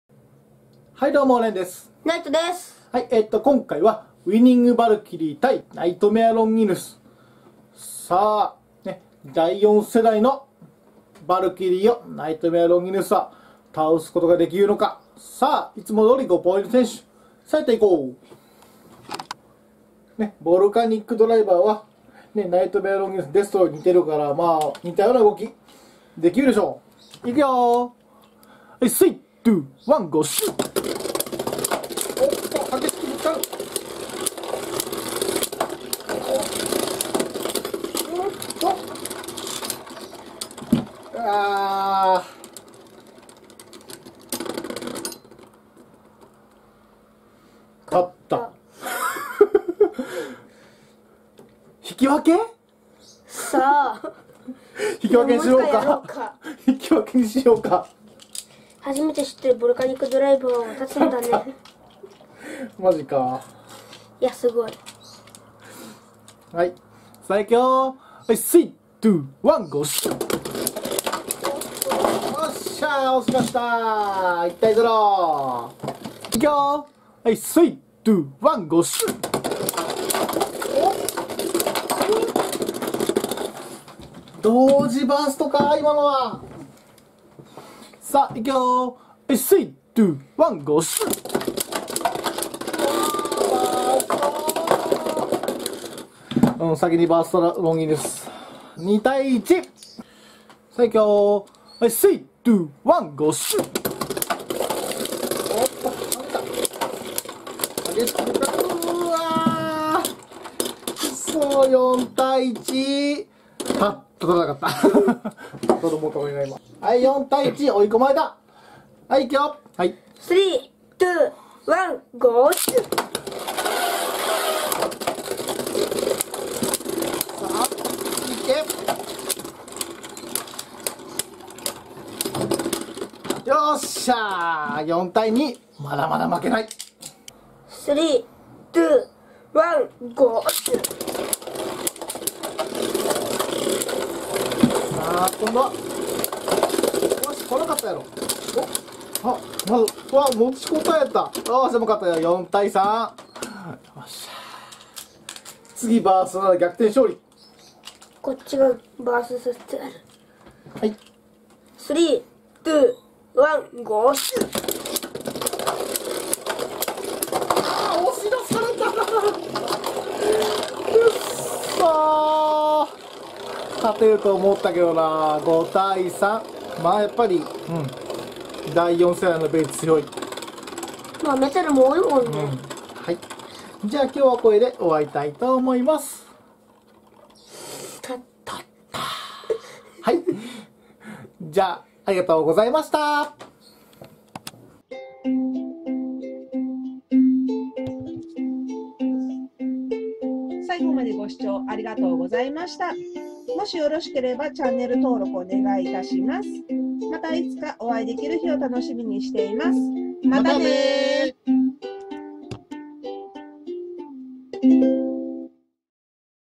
ナイトメアロンギヌスvsウィニングヴァルキリー 【ベイブレード】
さぁ第四世代がはじまります
新型ヴァルキリーのつよさはいかに？